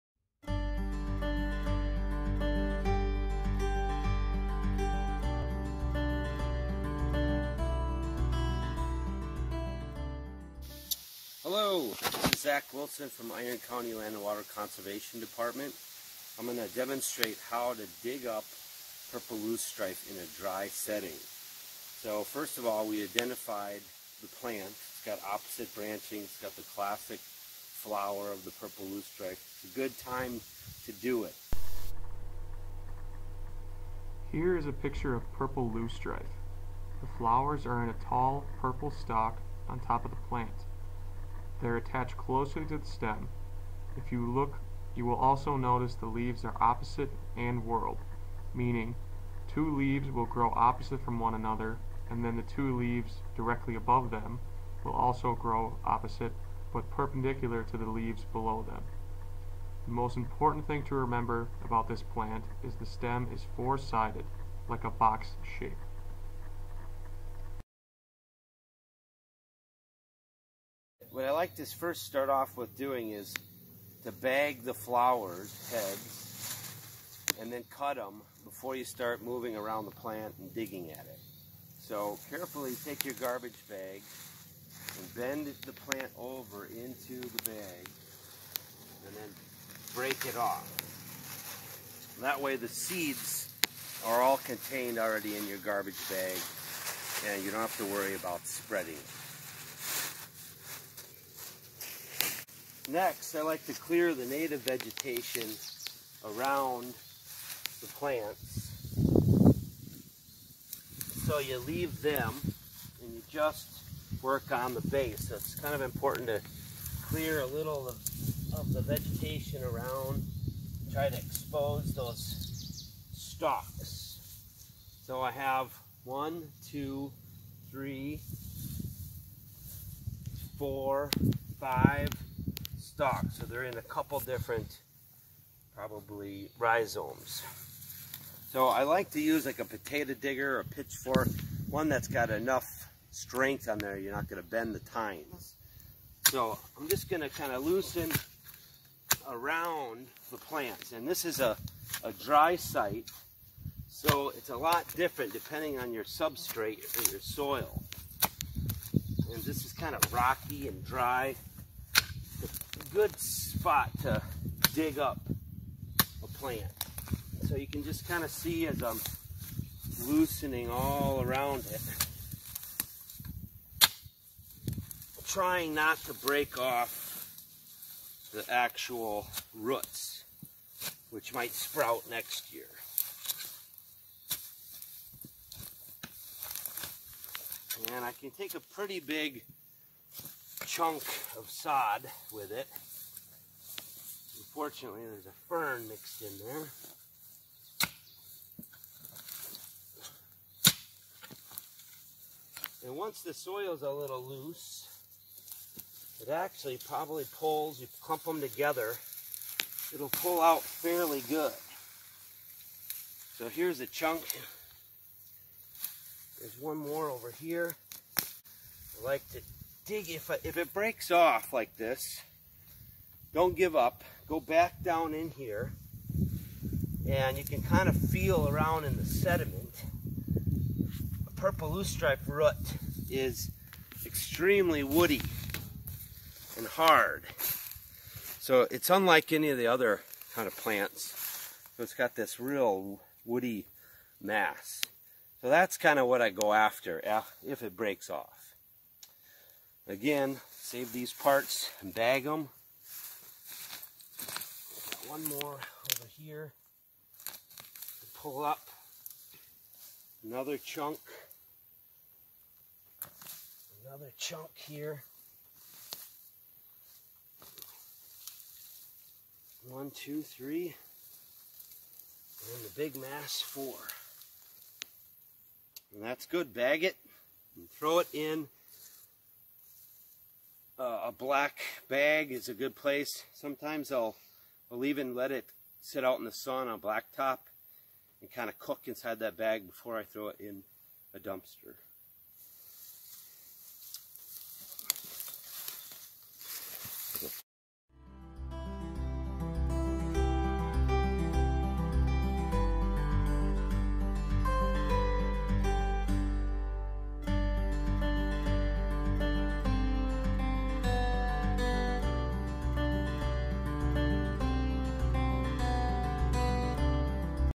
How to Remove Purple Loosestrife from Dry Sediment
The is a video starring Zach Wilson. In this Video Zach will teach us how to successfully remove Purple Loosestrife from a dry site.